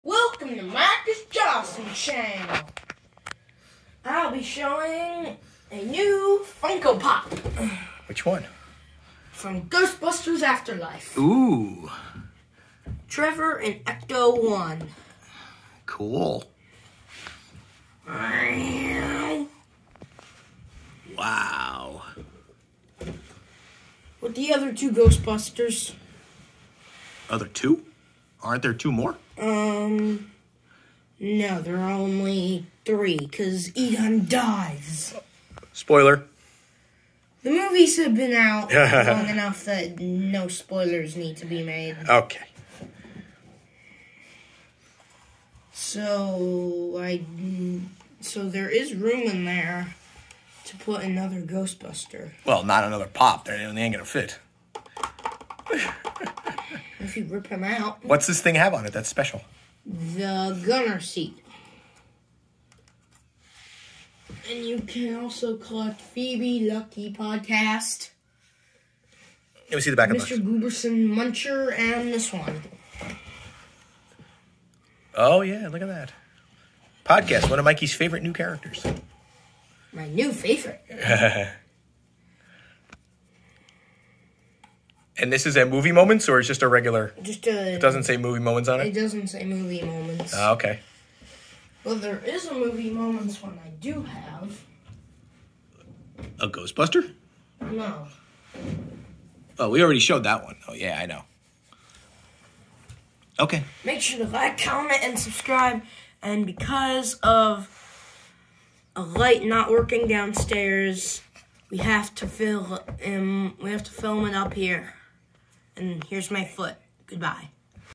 Trevor driving Ecto1 Funko from Ghostbusters-Afterlife
Here is a pretty cool scene of one of the newest Ghostbusters.